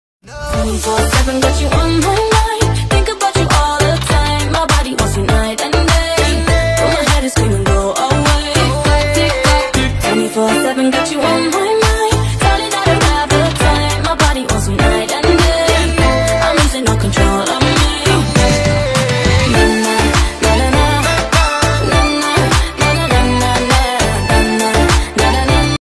Leyla Lydia's Some Makeup Clips
Very Cute And Beautiful Turkish actress Leyla Lydia Tugutlu is in happy mode.
Best Videos of Leyla Lydia Tugutlu are here in this channel